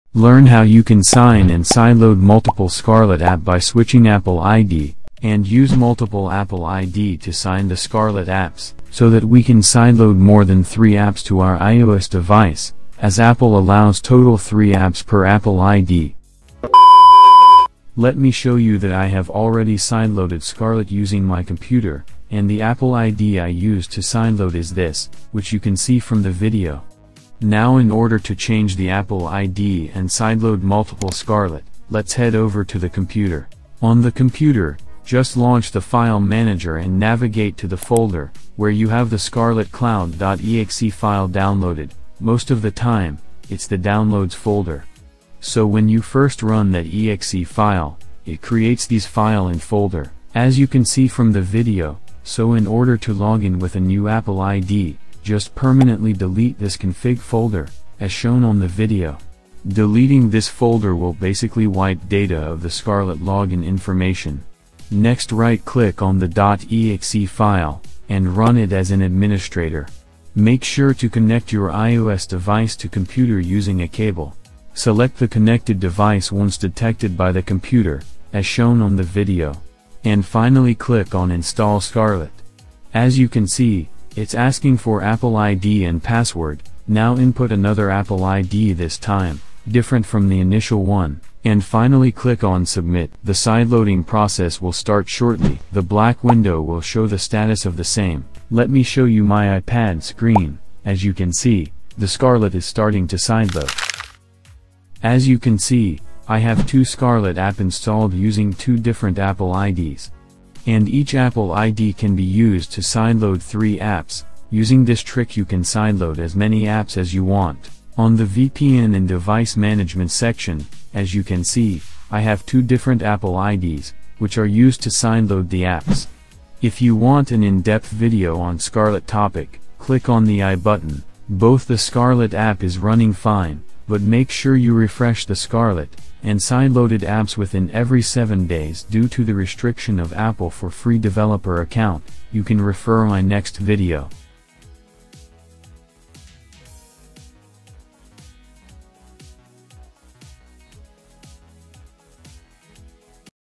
Clone Scarlet: Switch Apple ID with Scarlet iOS | Windows
Tired of the three-app limit on your free developer account? Say goodbye to restrictions and hello to endless possibilities. Learn how to change your Apple ID effortlessly, sideload multiple Scarlet apps, and remove the three-app limit for good.

Discover Scarlet's secrets, sign apps with different Apple IDs, and supercharge your iOS experience. Learn how to clone Scarlet, troubleshoot Scarlet iOS issues, and harness the power of Scarlet on Windows. Unveil the secrets of changing your Apple ID for Scarlet and sideload unlimited apps.

🔗 Text to Speech : voicemaker.in